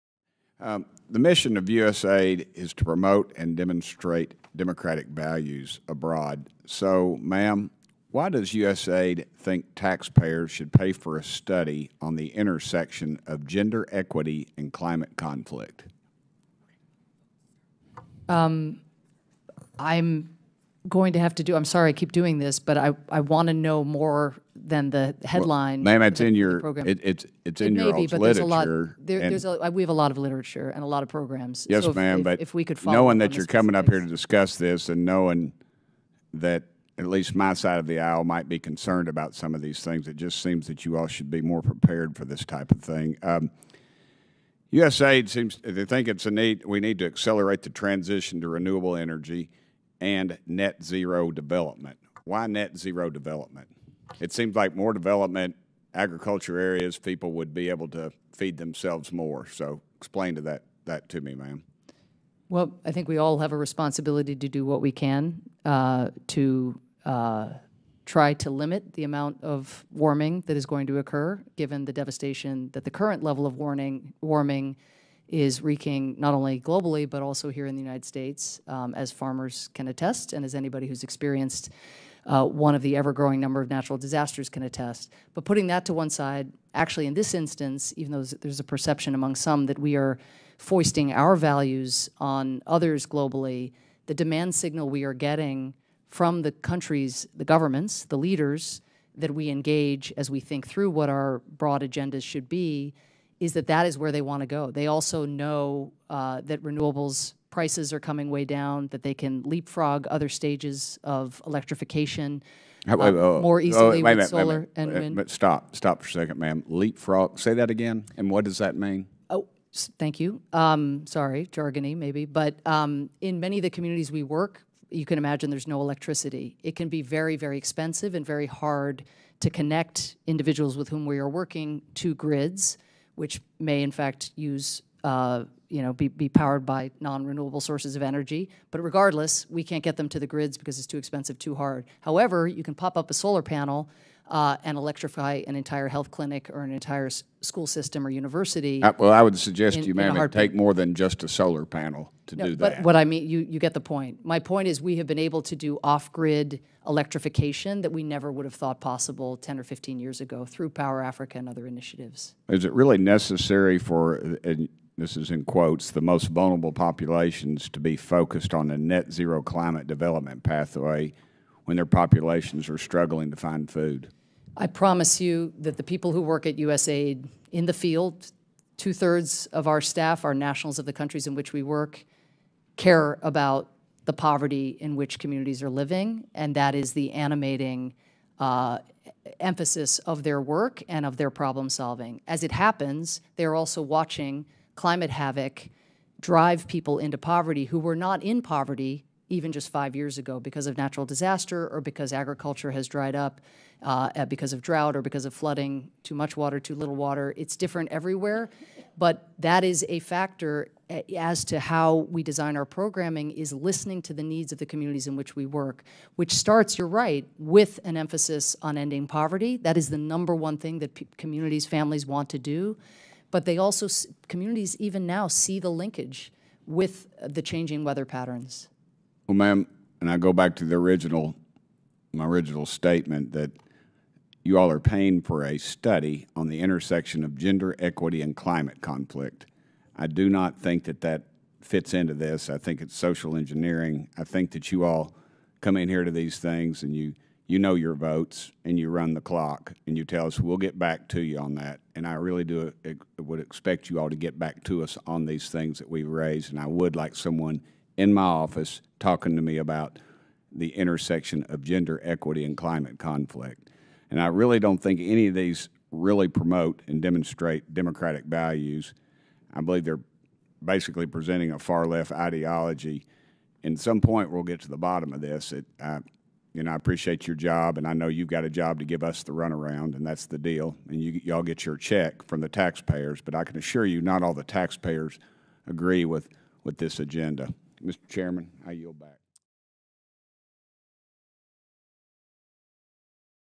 Rep. Burchett rips USAID administrator over gender equity priorities in third world countries
Congressman Tim Burchett (TN-02) questions USAID administrator on their gender equity and climate priorities in third world countries.